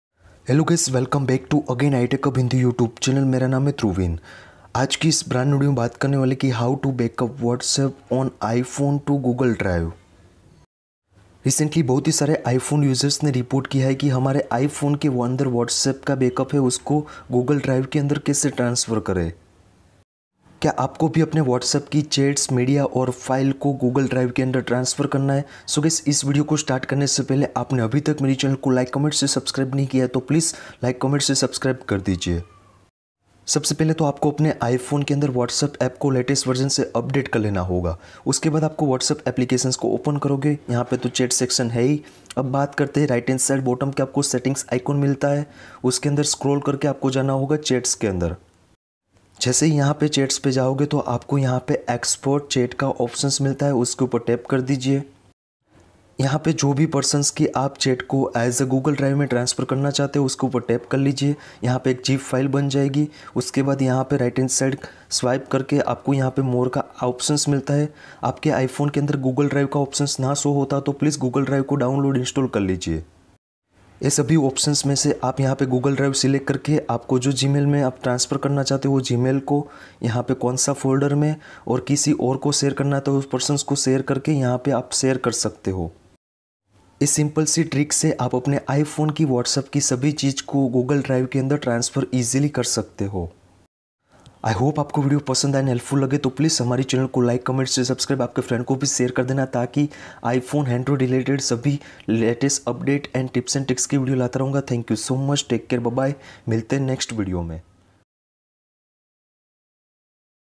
How to Whatsapp Back Up on iPhone to Google Drive (2024)
is video simple step se app whatsapp chat, media or document ka backup iphone se google drive me kaise transfer kare ?

is video pure end tak dekhna or support karna